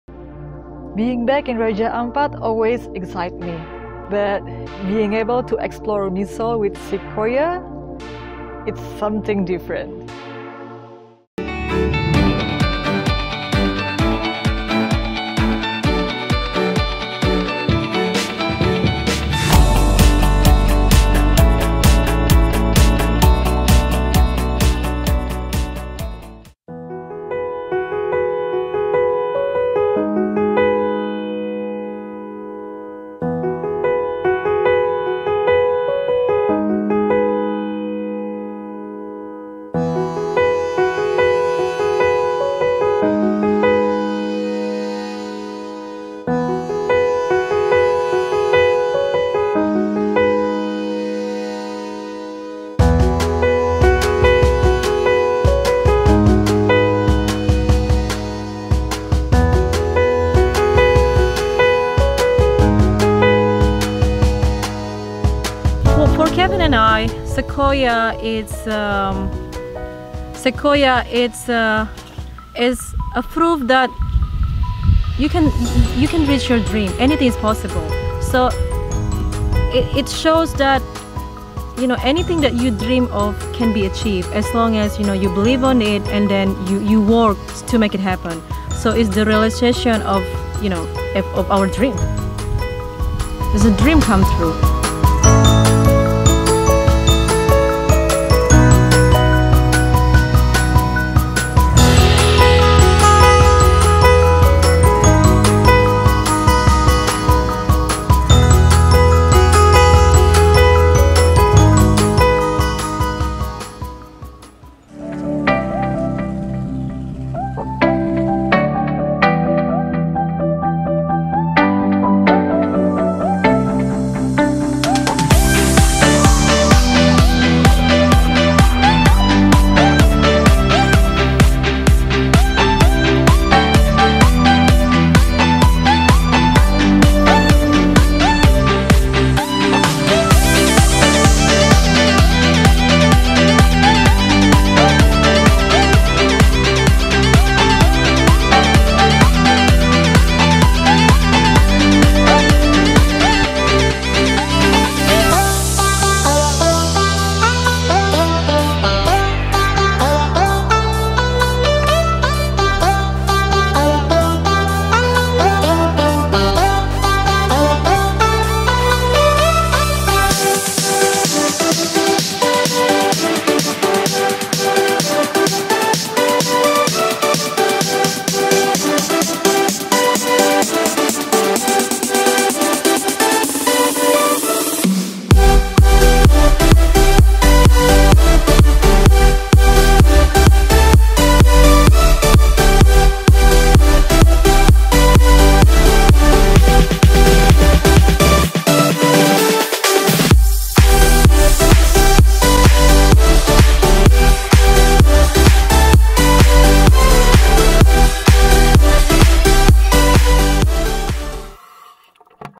Sequoia Luxury Yacht in Misool (intro)
Wet Traveler was invited to a trip in Misool by Sequoia Luxury Yacht.
It was a trip we will never forget.
This episode is an intro to our exciting journey with Sequoia Luxury Yacht.